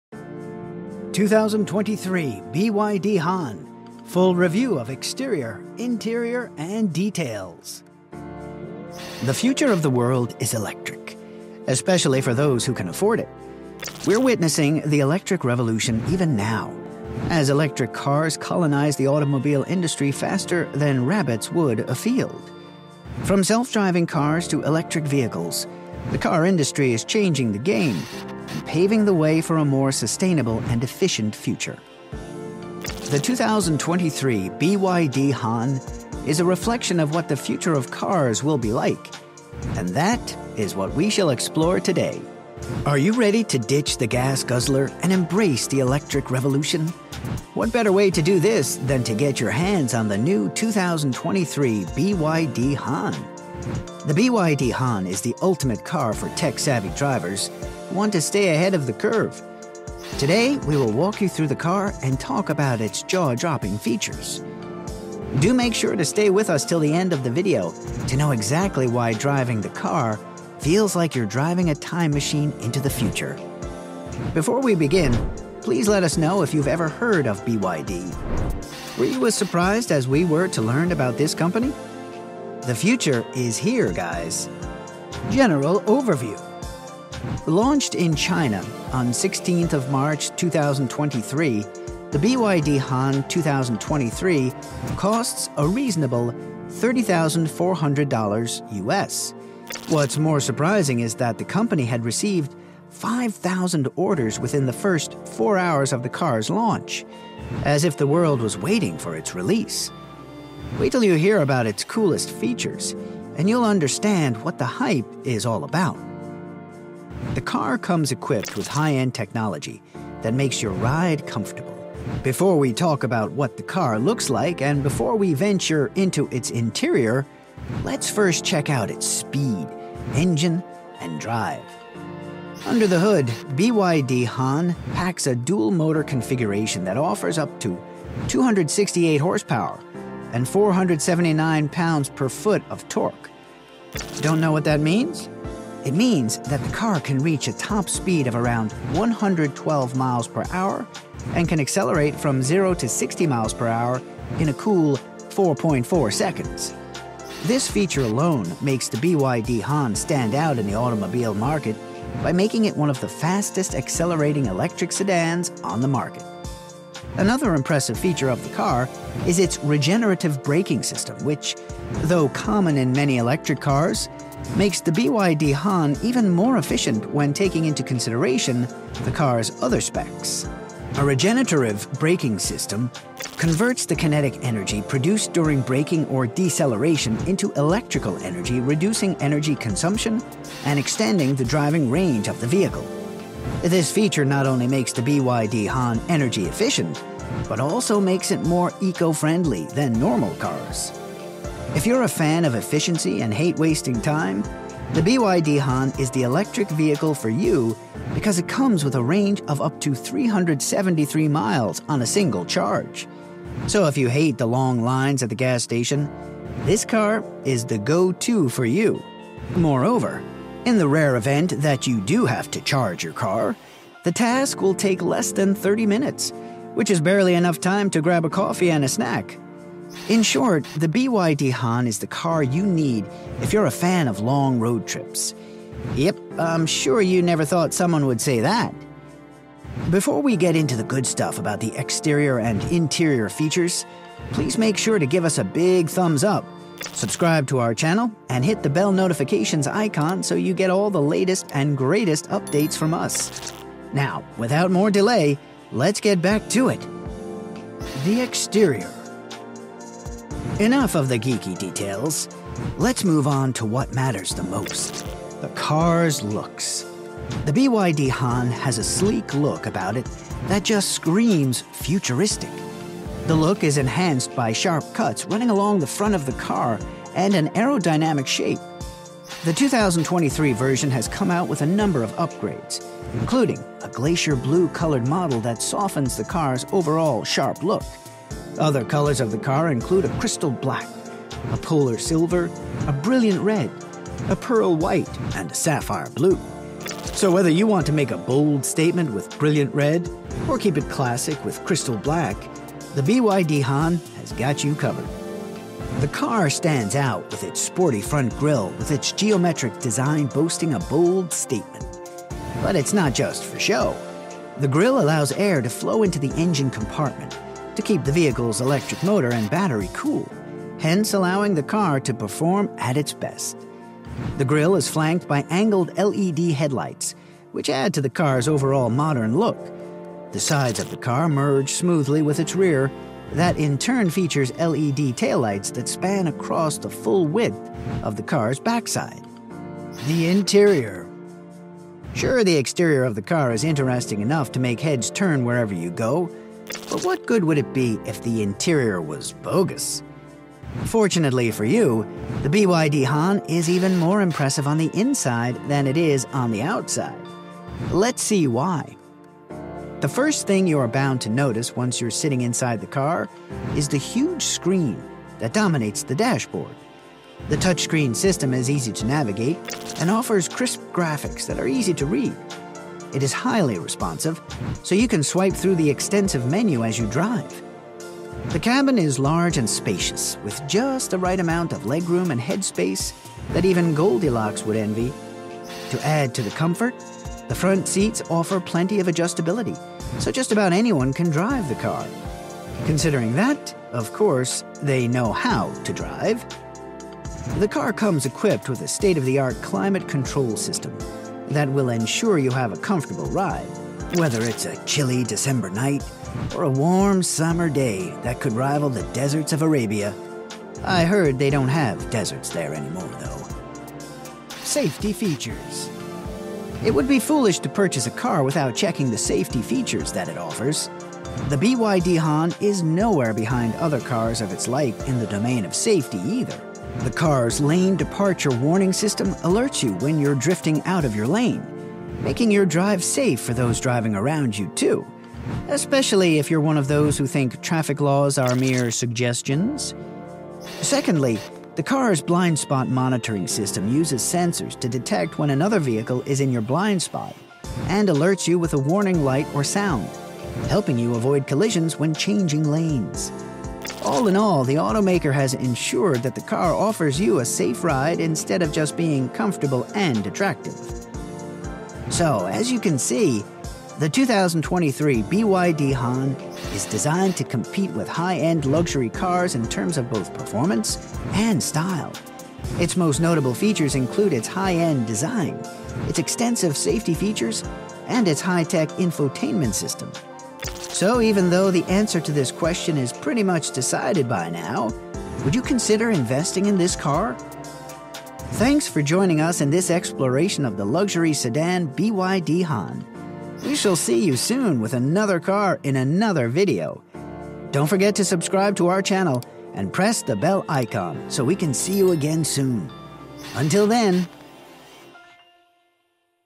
2023 BYD HAN Full Review of Exterior, Interior and Details!
In this video we take a look at the 2023 BYD HAN - Full Review of Exterior, Interior and Details!

The future of the world is electric, especially for those who can afford it. We are witnessing the electric revolution even now, as electric cars colonize the automobile industry faster than rabbits would a field. From self-driving cars to electric vehicles, the car industry is changing the game and paving the way for a more sustainable and efficient future. The 2023 BYD HAN is a reflection of what the future of cars will be like, and that is what we shall explore today!

Be kept in the loop on all of our latest videos.

Sending you love and prosperity,
 Grand Luxury